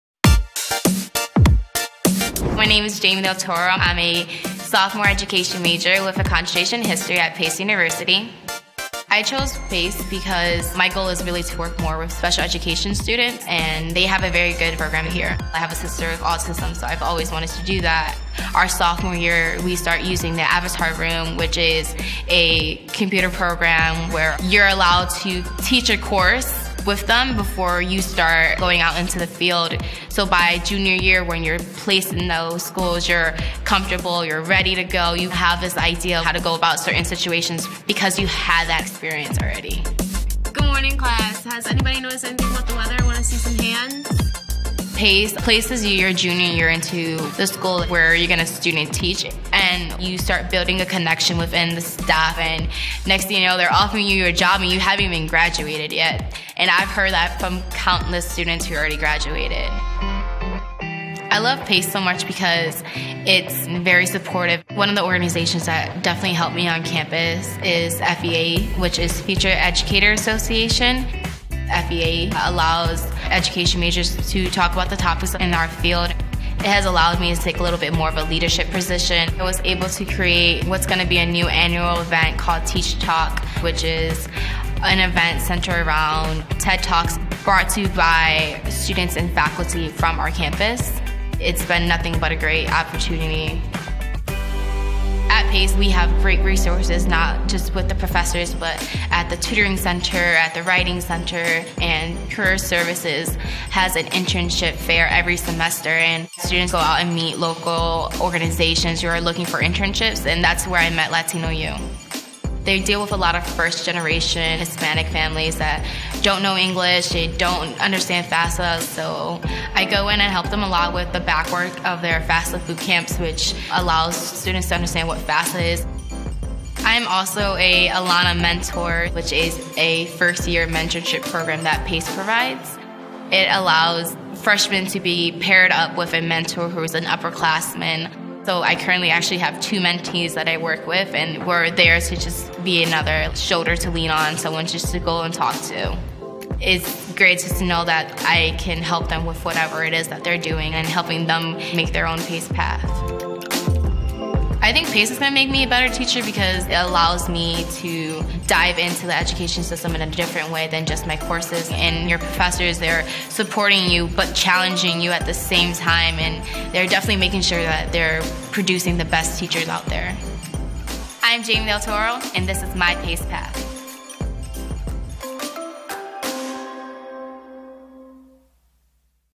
Student, Jamie DelToro and her Pace Path
Jamie's commitment to becoming a teacher stems from the positive impact that her teachers had on her—she wants to make a difference in the lives of children, too. By studying at Pace, Jamie is getting the education and support she needs to do just that. Her internship, mentoring, and leadership roles, and the opportunity to learn teaching techniques before ever stepping into a classroom are giving her an invaluable real-world understanding of her chosen career.